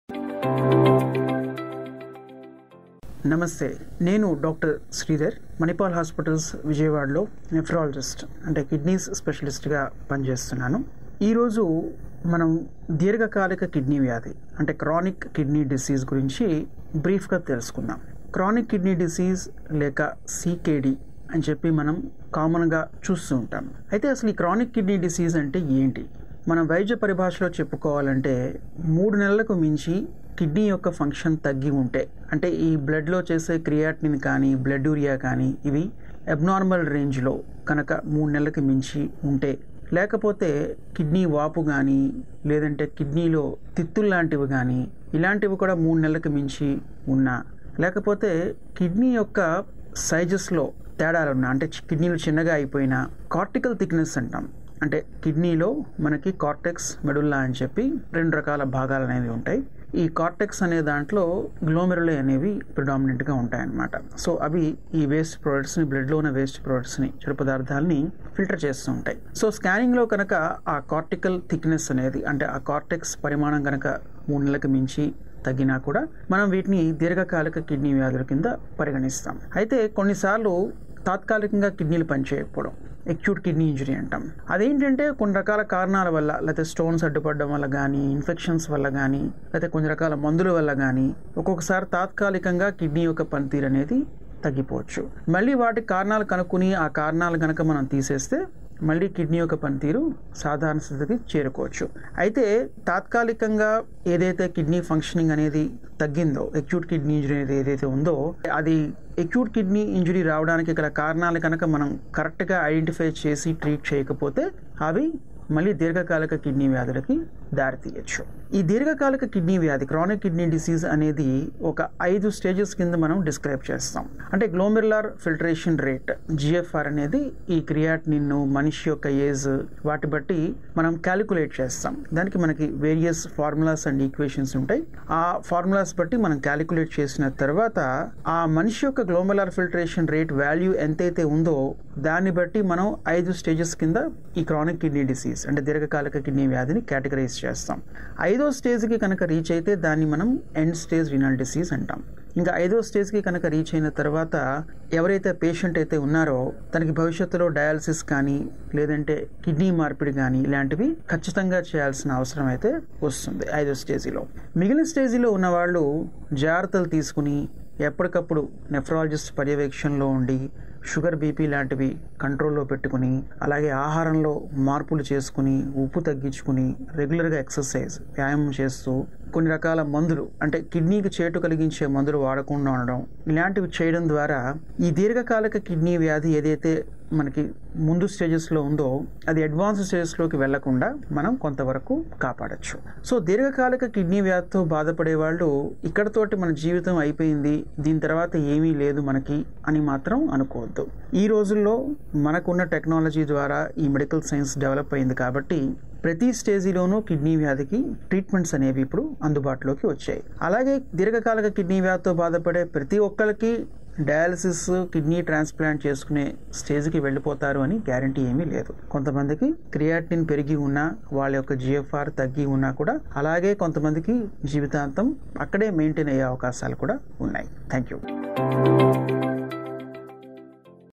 Chronic Kidney Disease | Dr. Sridhar A.V.S.S.N | Kidney specialist in Vijayawada -Manipal Hospitals
Chronic Kidney Disease (CKD) develops as a result of damage to the kidneys that prevent them from filtering the blood as efficiently as they should. Dr. Sridhar A.V.S.S.N, Consultant - Nephrology and Renal Transplant Physician, Manipal Hospital Vijayawada, speaks about this condition, its causes and treatment.